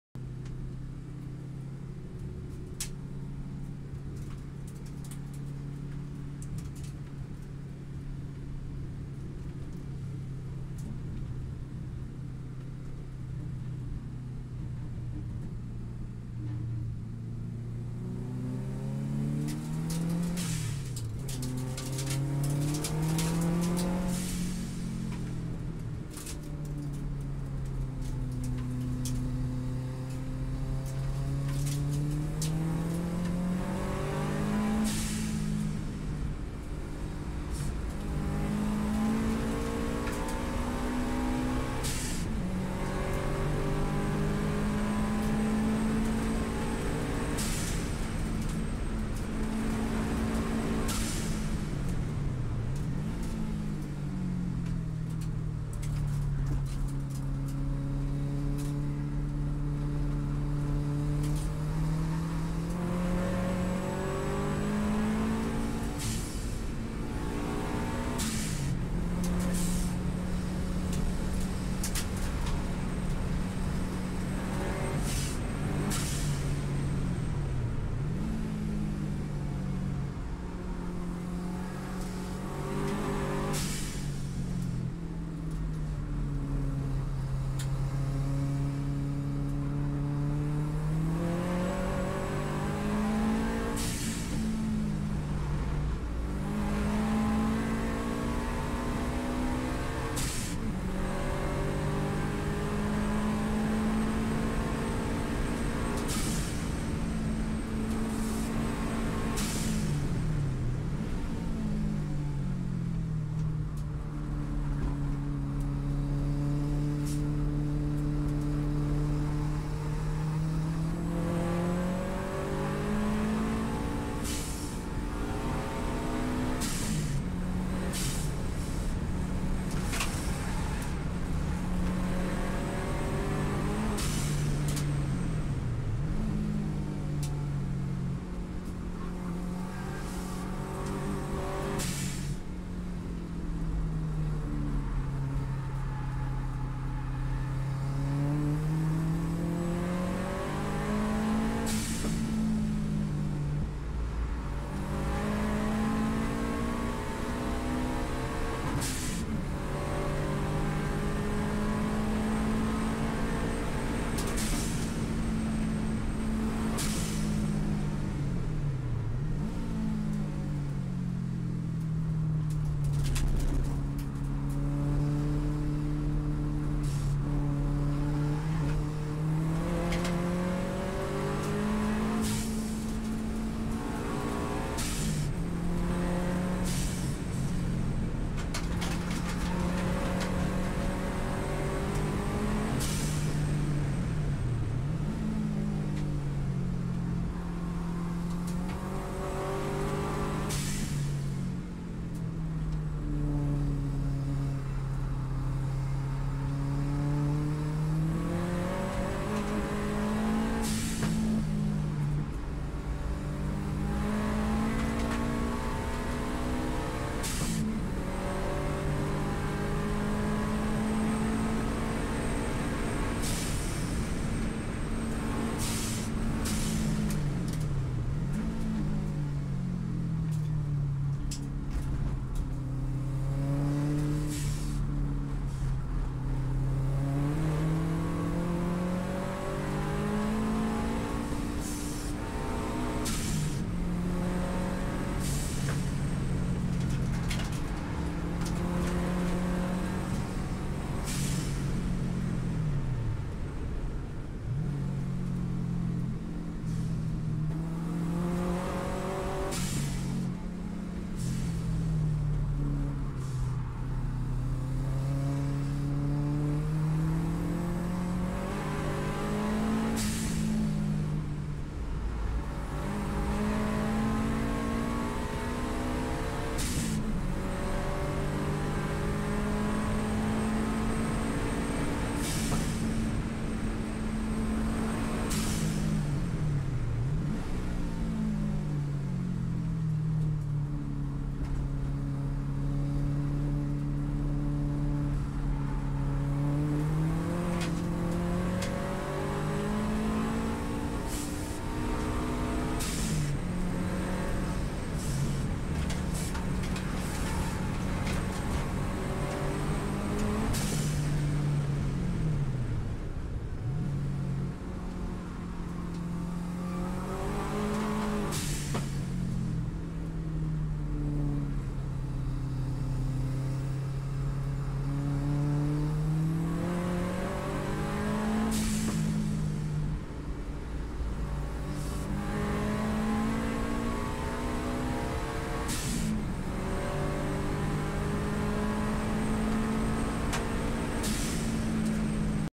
Subaru Sti Vs V8 Ford Escort at lakeside raceway QLD 2012 Garry Ford & Nick Contojohn
Lakeside raceway QLD a few weekends ago, fun day, the first time i had seen the track.
managed to get 2nd outright for the day with a time of 58.31sec, Garry Ford out front trying to keep with him, a little to fast for me, Garry was fist outright with a time of 57.72